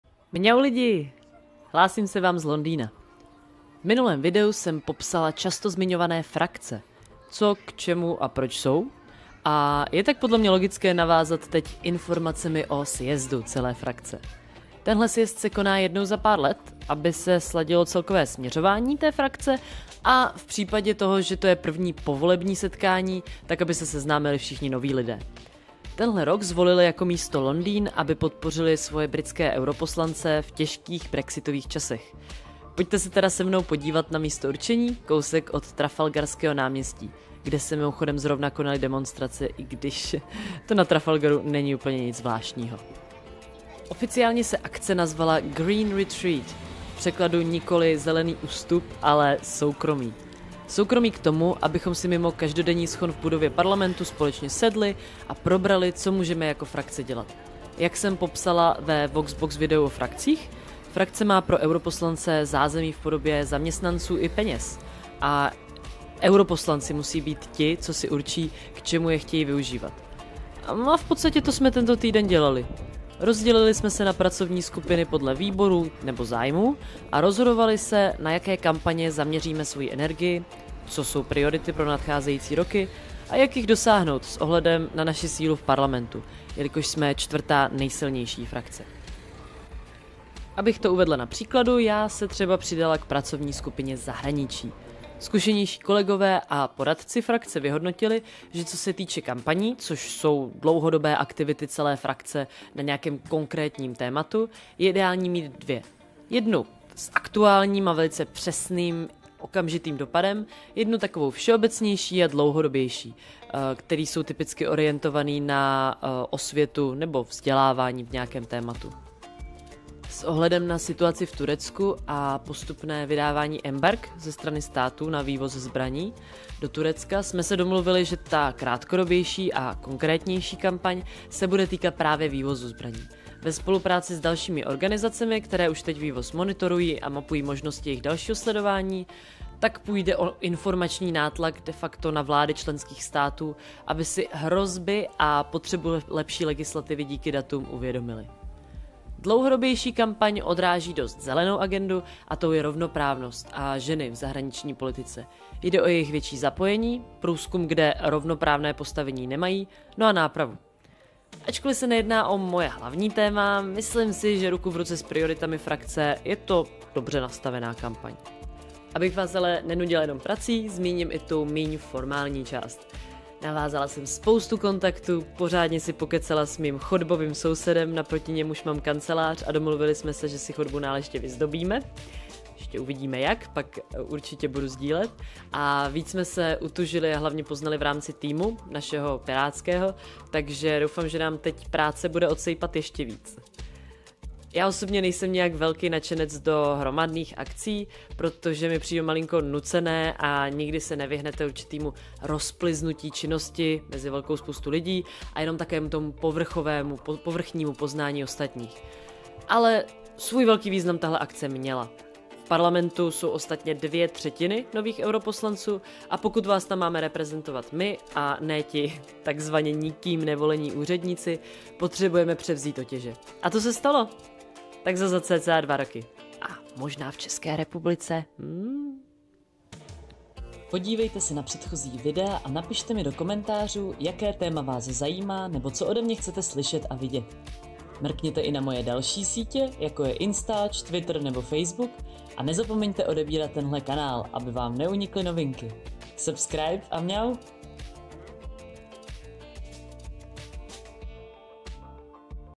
Tour po Londýně aneb Zasedání naší frakce [#8 BEHIND THE SCENES]
Doprovoďte mě na každoroční zasedání naší frakce londýnskými ulicemi a já vám po cestě povím, co jsme tam vlastně na začátku týdne dělali a k čemu to je. :)

Díky, že mě sledujete!